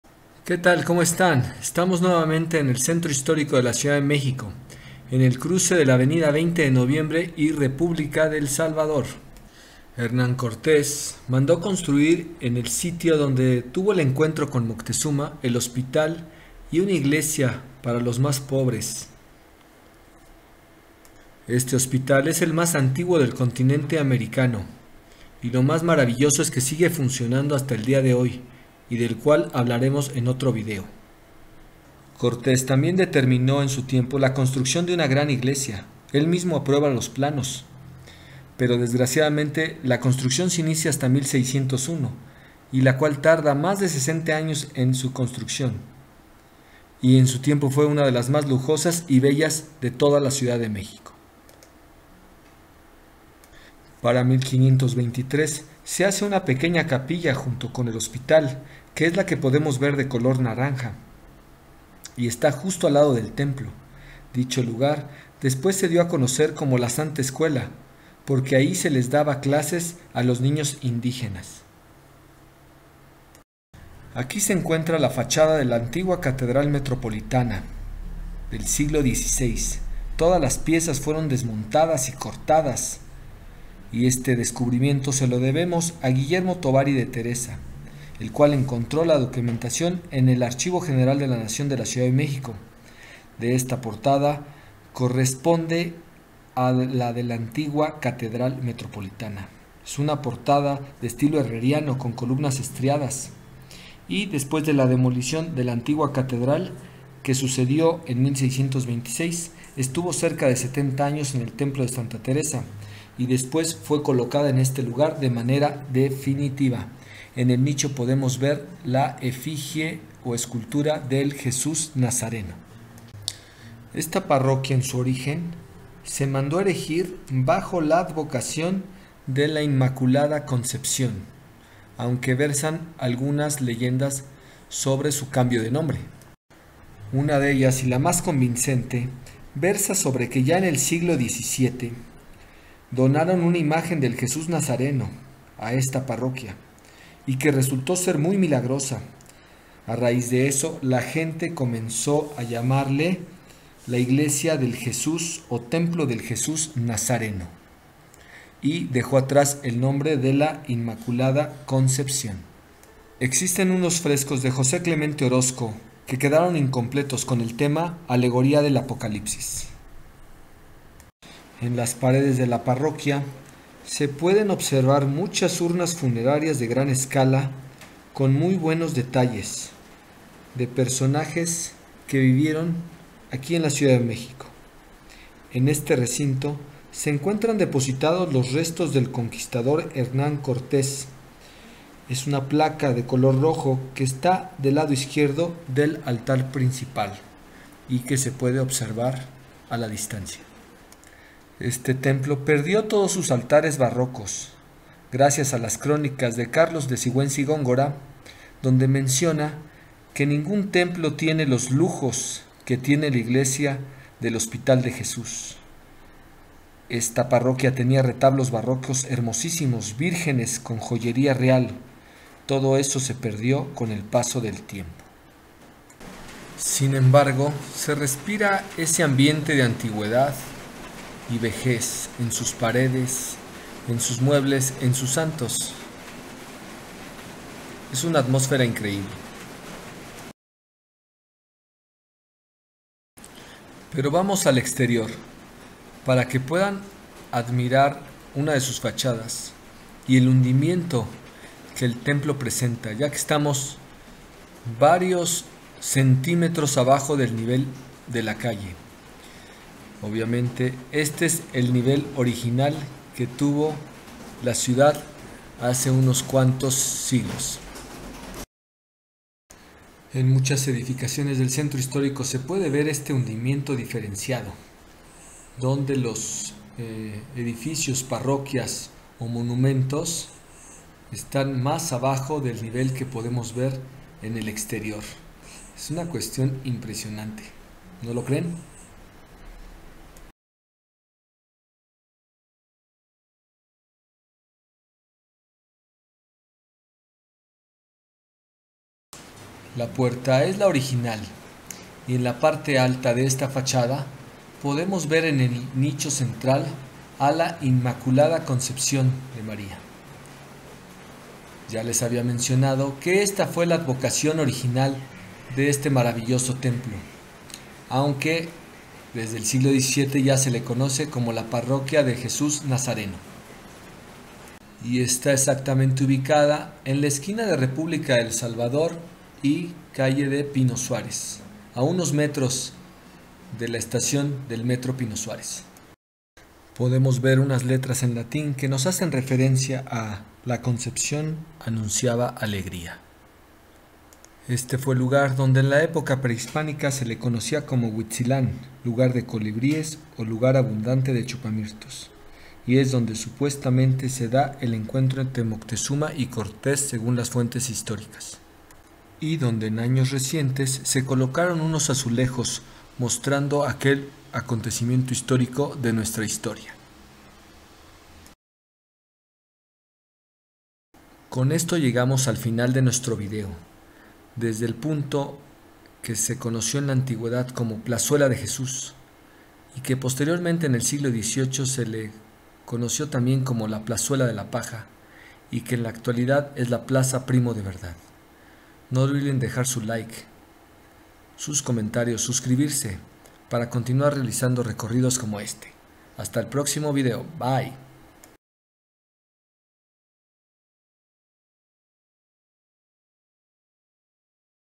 Parroquia donde descansan los restos de Hernán Cortés en le centro Histórico de la CDMX.
Visita a la parroquia de la inmaculada concepción, que en la actualidad se le conoce como la parroquia de Jesús Nazareno en el centro histórico de la Ciudad de México y donde descansan los restos del conquistador español Hernán Cortés.